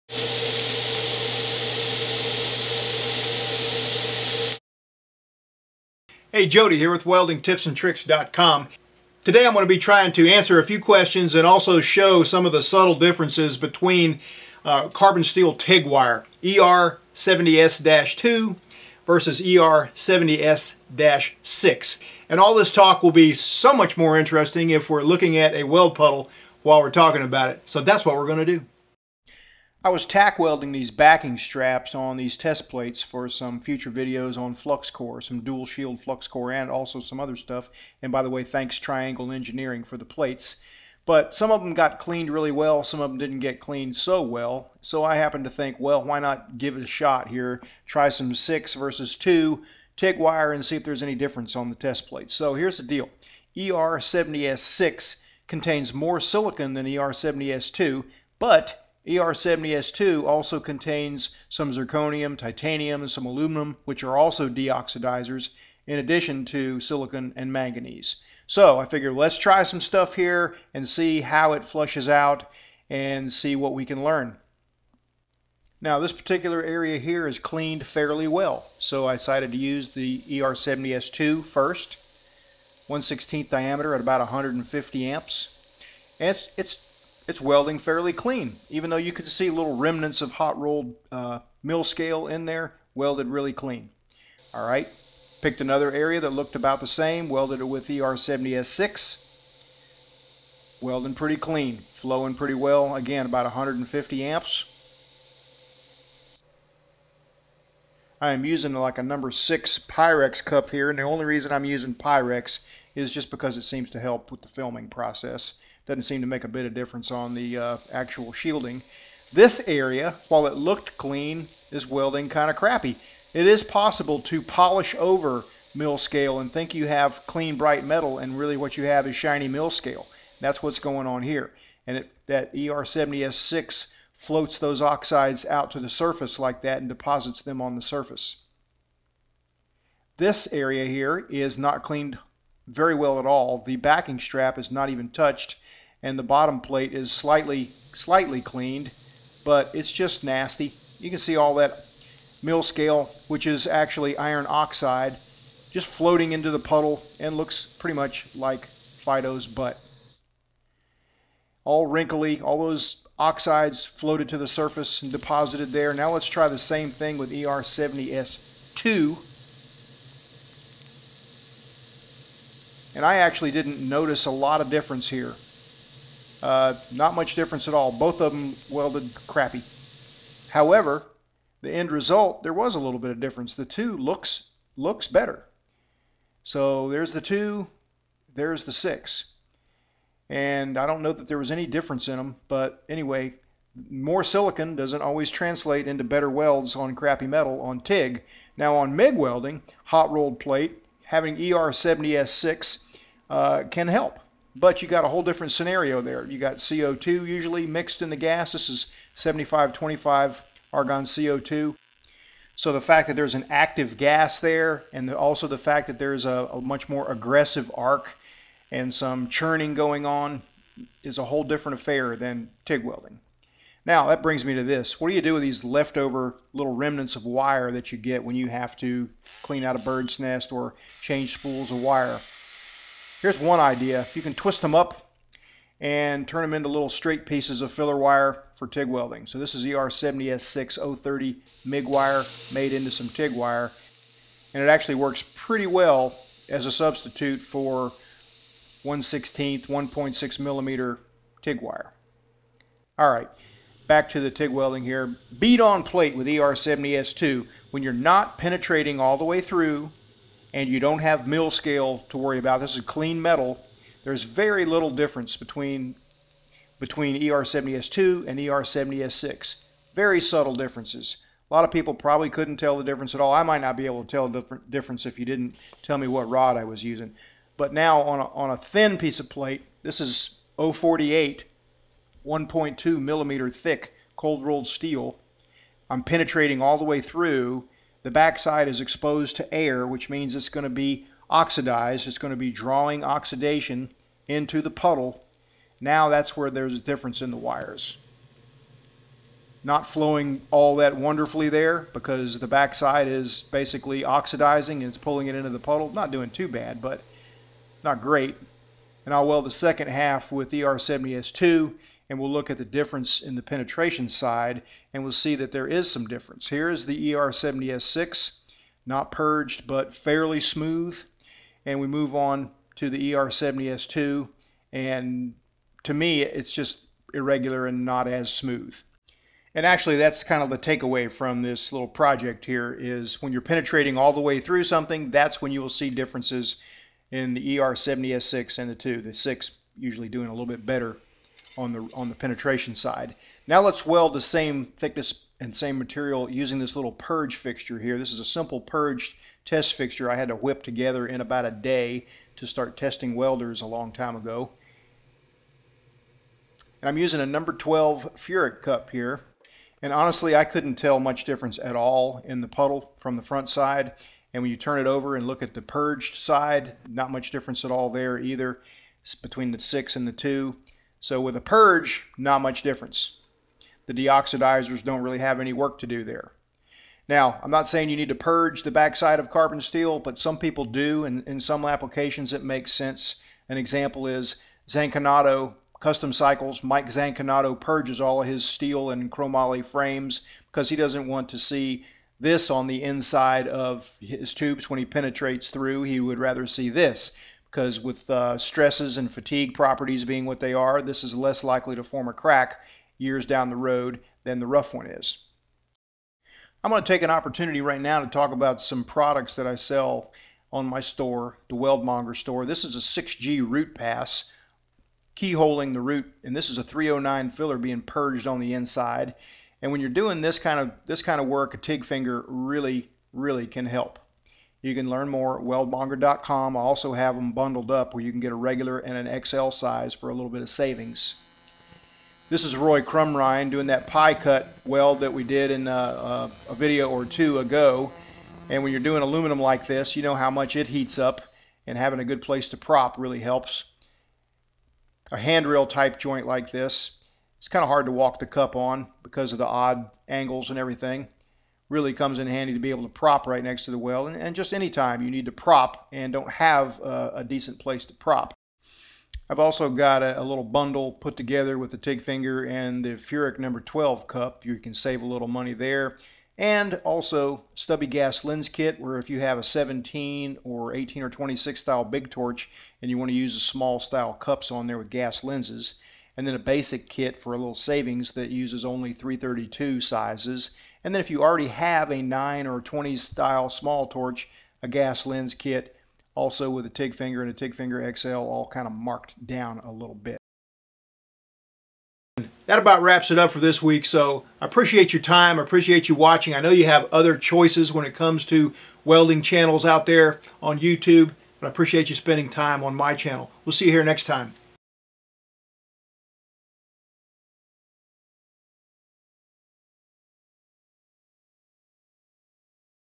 Tig Welding with ER70S-2 vs ER70S-6
While I was tack welding some hot rolled steel plates, I decided to experiment with er70s2 and er70s6 Tig Filler metal. For me the differences have always been subtle.  But I did discover one situation where er70s6 seemed to perform better.